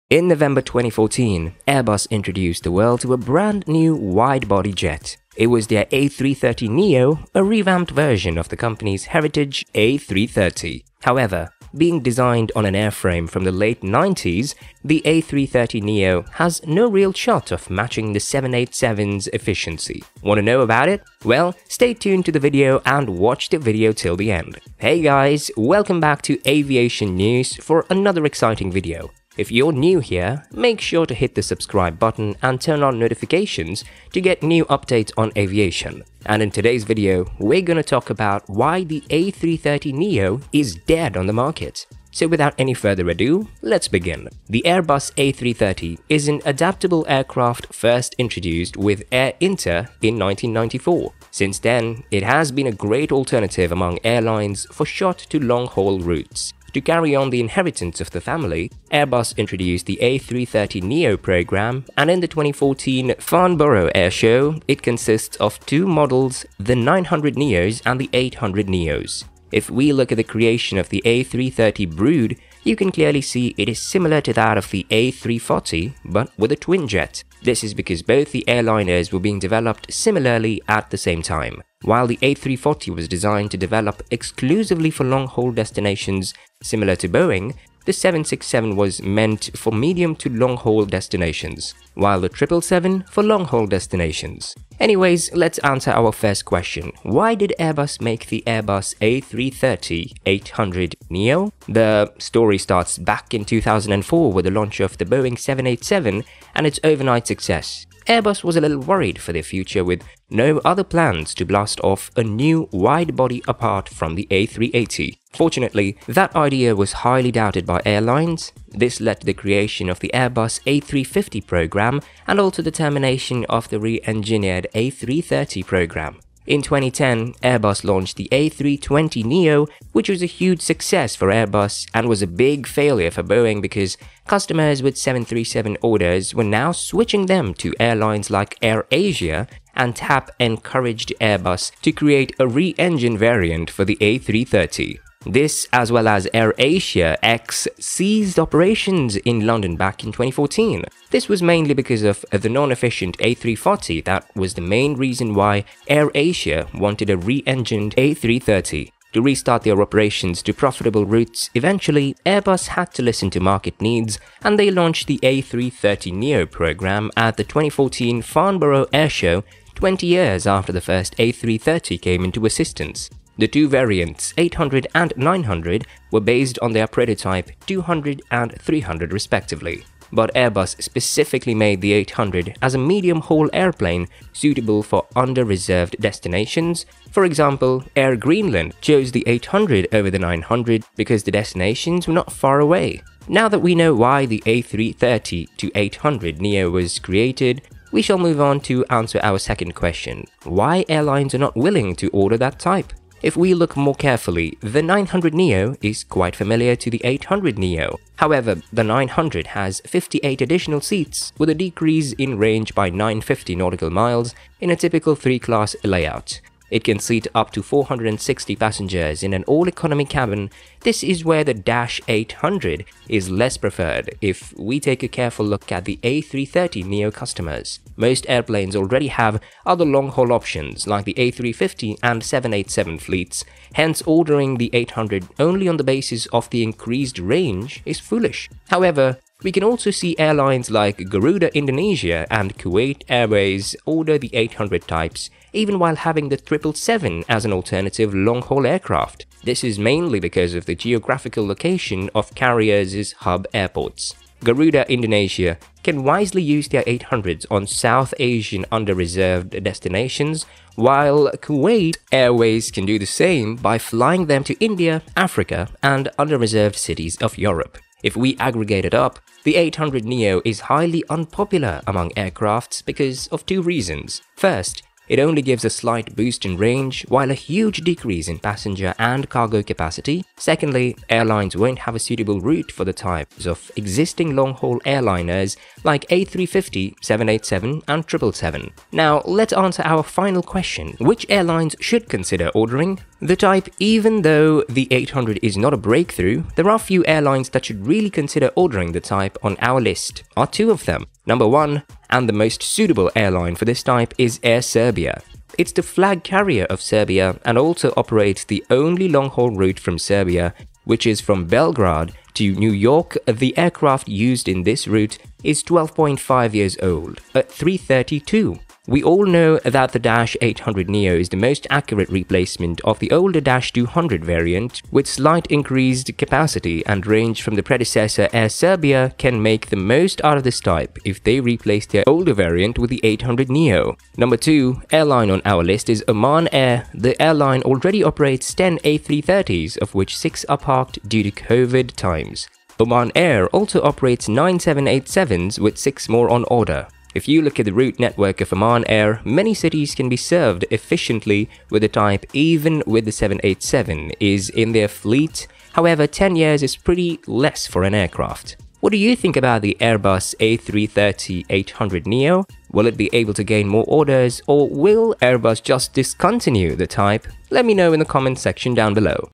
Why Airbus A330neo Is So SUCCESFULL!
Welcome by Aviation News!

In November 2014 Airbus introduced the world to a brand-new wide-body jet. It was their a330 neo, a revamped version of the company's heritage a330. However, being designed on an airframe from the late 90s the A three-thirty neo has no real shot of matching the 787's efficiency. In today's video, we are going to talk about "Why is the A330Neo is dead on the market".